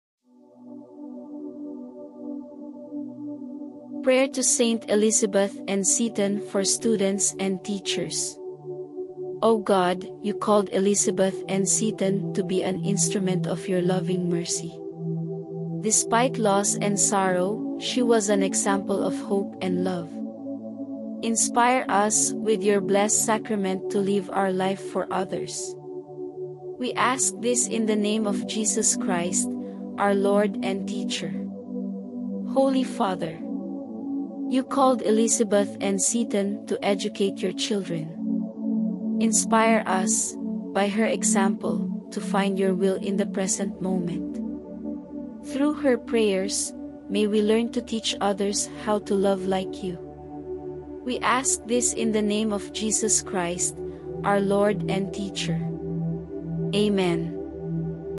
A Prayer to ST. ELIZABETH ANN SETON for Students and Teachers
Saint Elizabeth Ann Seton Prayer for Students

O God,
You called Elizabeth Ann Seton to be an instrument of your loving mercy.
Despite loss and sorrow, she was an example of hope and love.
Inspire us with your Blessed Sacrament to live our life for others.
We ask this in the name of Jesus Christ, our Lord and Teacher.
Amen.

Saint Elizabeth Ann Seton Prayer for Teachers

Holy Father,
You called Elizabeth Ann Seton to educate your children.
Inspire us, by her example, to find your will in the present moment.
Through her prayers, may we learn to teach others how to love like you.
Amen.

🌹 MORE ABOUT OF ST. ELIZABETH ANN SETON🌹

St. Elizabeth Ann Seton was born in New York City in 1774. Her family was very rich, and they were part of high society. St. Elizabeth grew up Episcopalian and benefited from the best things in life due to her family’s status.

One of those benefits was learning how to read. It is noted the St. Elizabeth grew up reading the Bible and was very well learned.

Tragedy struck St.
Tragedy struck St. Elizabeth’s family early in her life. Her mother died when she was three. A sister died the following year. Still, St. Elizabeth remained cheerful. Her father ensured she grew up learning how to serve others.

As part of a rich family, St. Elizabeth was a popular young woman. Many eligible men wanted to marry her. She was married at age 19 to William Seton. He also came from a wealthy family. They had a happy marriage and loved each other dearly.

The Setons had five biological children. They also cared for William’s seven half brothers and sisters after William’s father died.

Soon after taking on this responsibility, William’s business went bankrupt. At the same time, he contracted tuberculosis. The Setons traveled to Italy to try to save his health. It was too late. St. Elizabeth’s beloved husband died, leaving her alone and poor.

Most people would have rejected God throughout these trials. But the struggles only made St. Elizabeth draw closer to God.

During her time in Italy, St. Elizabeth was introduced to Catholicism. Three things in particular attracted her to the faith: the Eucharist, Mary, and the faith’s history back to the Apostles.

When St. Elizabeth returned to the United States with her family, she became a member of the Catholic Church in 1805. This decision caused many of her family members and friends to abandon her. However, her joy in the faith was stronger than their disapproval.

She and her family moved to Baltimore. She decided to open a school there as a way to support her family financially. The school eventually became a Catholic school. It was the first free Catholic school in the country.

St. Elizabeth’s friends, who ran the school with her, decided to create a religious order. St. Elizabeth joined this new order as well, with amendments made so she could take care of her family.

Within a decade, this order had grown to two schools and two orphanages. St. Elizabeth eventually became the head of the order. It would continue to grow, but she soon contracted tuberculosis. After a period of fighting through illness, St. Elizabeth died in 1821. She was canonized in 1975.

St. Elizabeth is the patron saint of widows, orphans, and those who have difficult relationships with their in-laws.

Saint Elizabeth Ann Seton’s Feast Day: January 4

Hello everyone! I hope everyone is feeling great. The Feast of ST. ELIZABETH ANN SETON is celebrated on January 04, 2024.

❣❣❣❣ Credits / Sources ❣❣❣❣

Then Jesus said, “Did I not tell you that if you believe, you will see the glory of God?” John 11:40